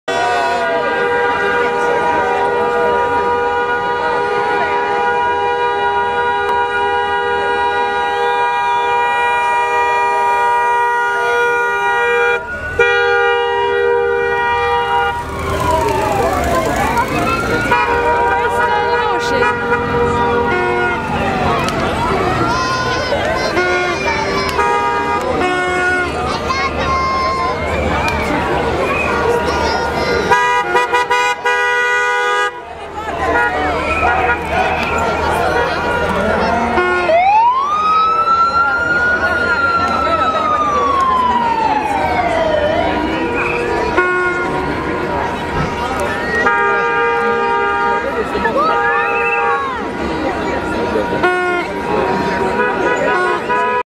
Saint Patrick's Day, Cork-IRL 2011 (1)
Saint Patrick's Day in Cork-Ireland; 17MAR2011. Motocyclist ride on Grand Parade Street.